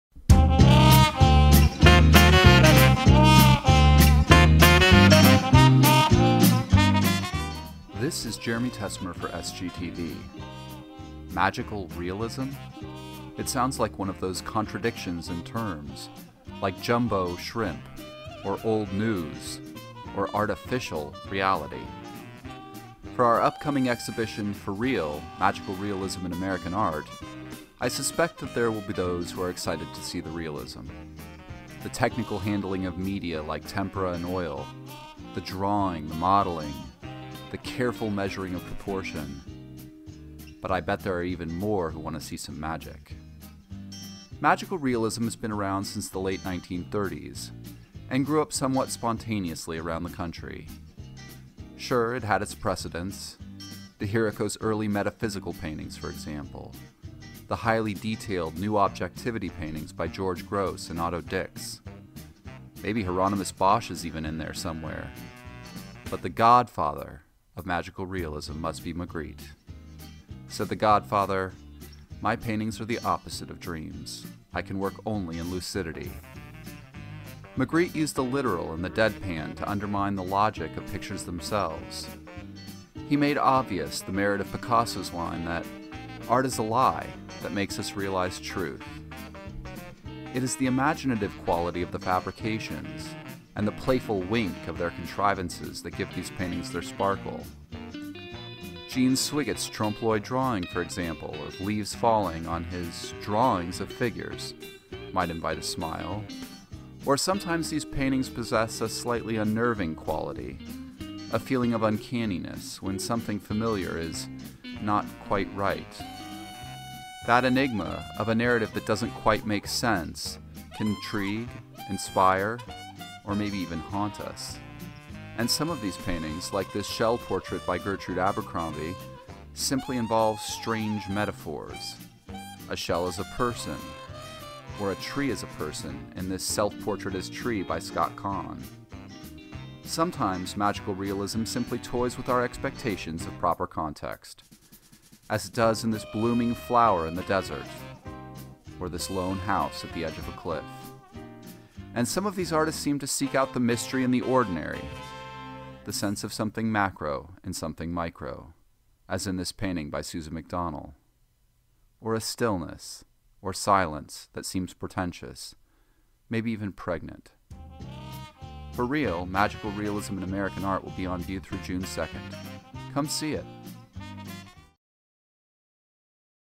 SGTV Presents FOR REAL? MAGICAL REALISM IN AMERICAN ART
Sullivan Goss presents an exhibition of American art made in the Magical Realist tradition with works by Gertrude Abercrombie, Mark Beck, Dorothy Churchill-Johnson, Richard Haines, Scott Kahn, Steven Kenny, Frank Kirk, Dan Lydersen, Susan McDonnell, Jean Donald Swiggett, Frank Taira, andJohn Wilde